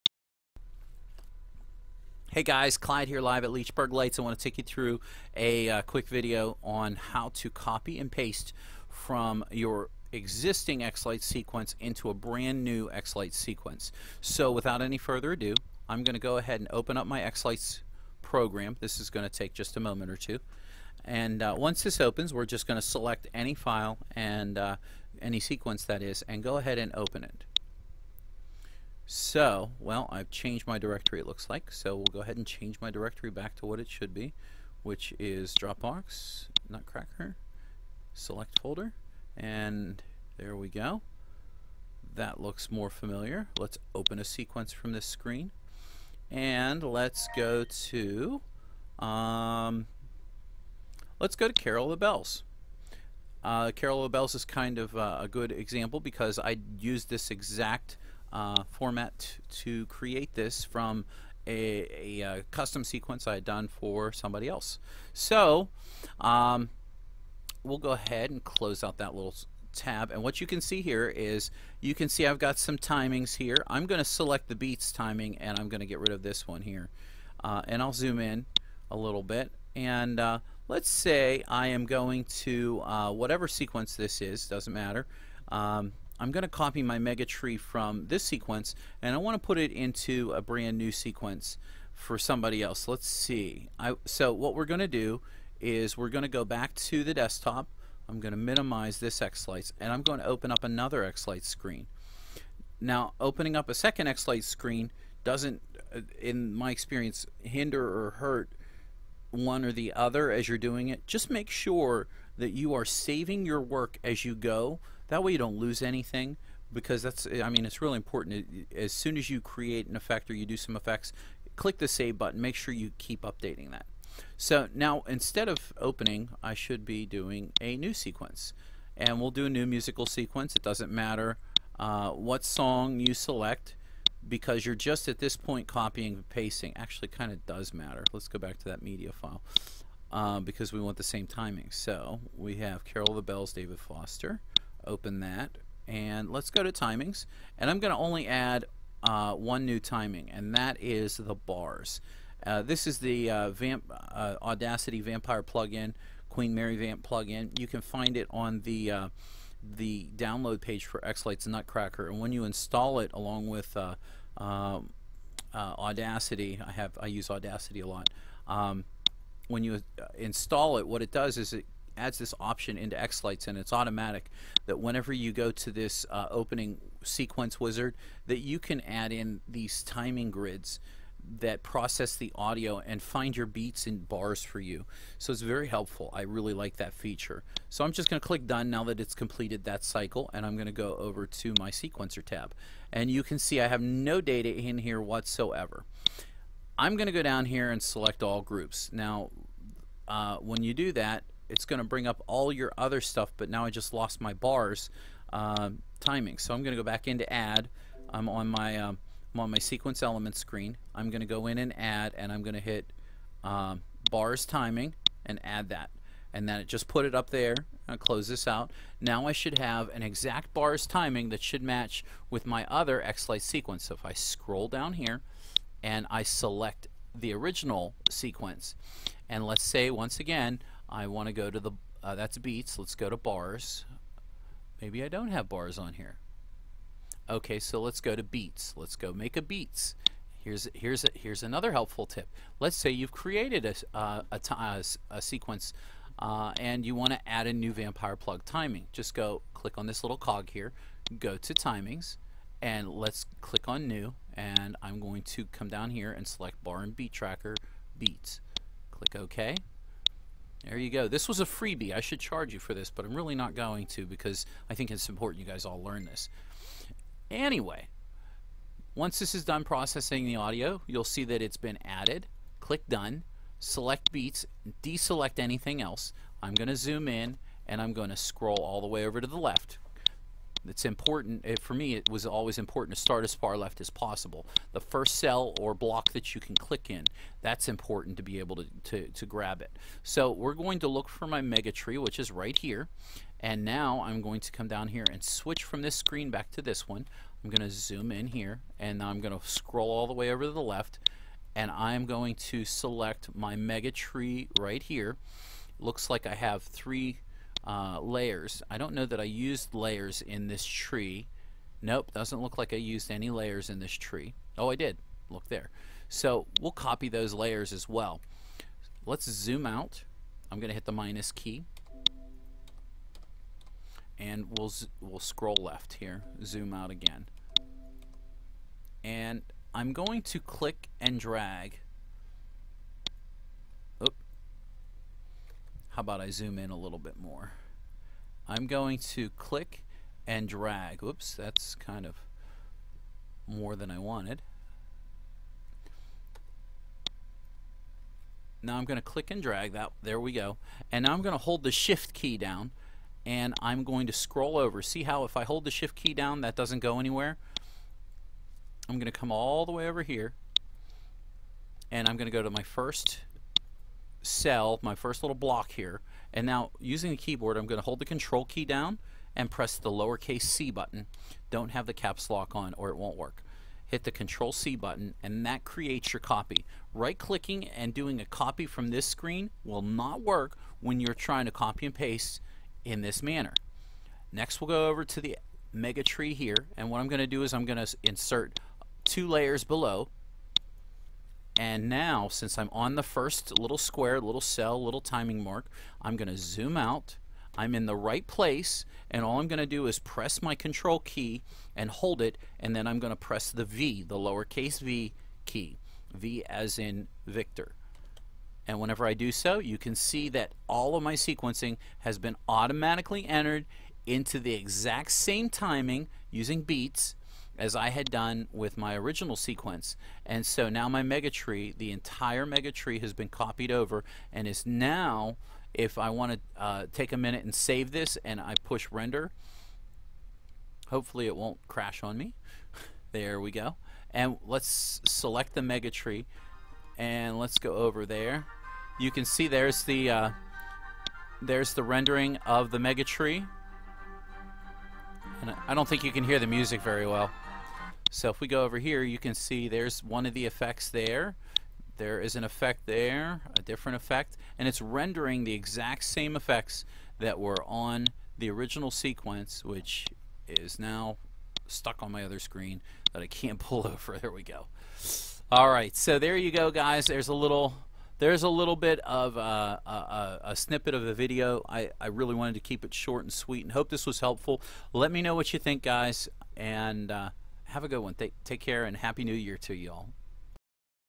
Copy Xlights 4 effects in to a Second XL4 Sequence
This video shows how to copy and paste sequenced effects from an existing XL4 sequence, then past it into a new XL4 sequence.